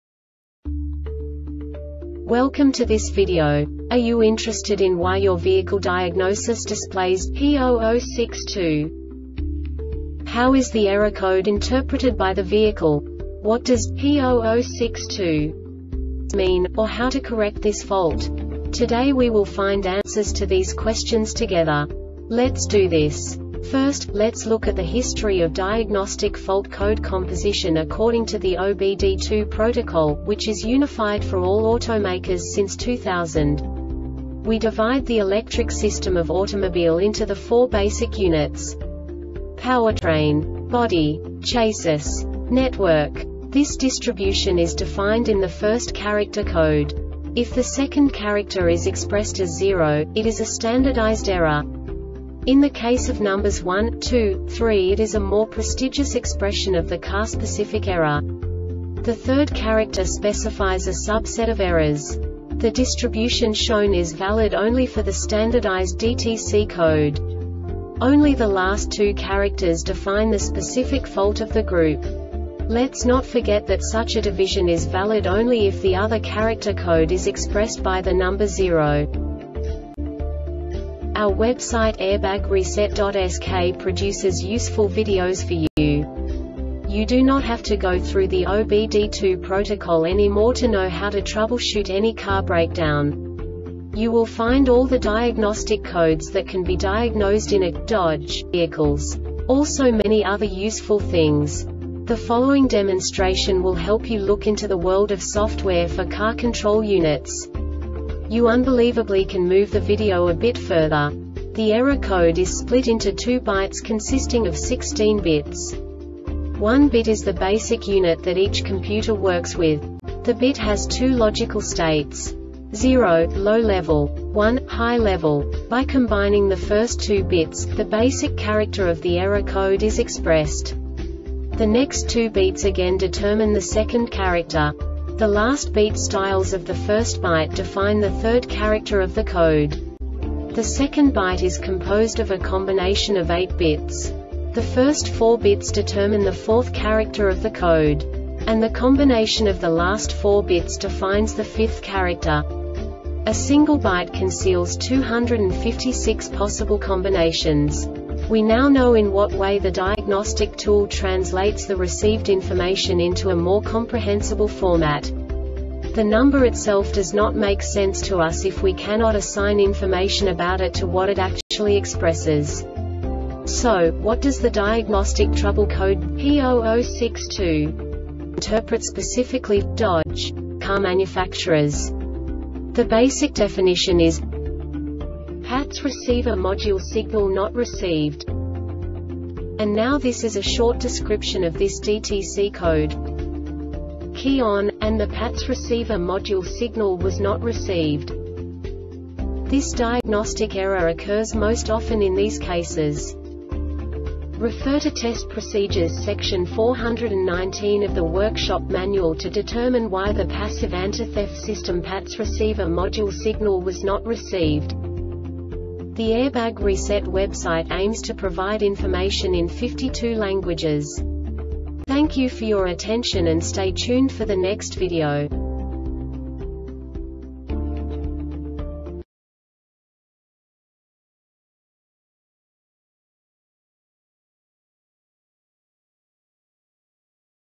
DTC Dodge P0062 Short Explanation
Contents:
0:21 Basic DTC analysis according to OBD2 protocol standard.
1:48 Insight into programming
2:58 A diagnostically understandable description of the Diagnostic Trouble Code
3:05 Basic error definition

Make
Dodge
Code
P0062
Definition
PATS Receiver Module Signal Not Received
Description
Key on,  and the PATS Receiver Module Signal was not received
Cause
Refer to test procedures Section 419 of the Workshop Manual to determine why the Passive Antitheft System PATS Receiver Module Signal was not received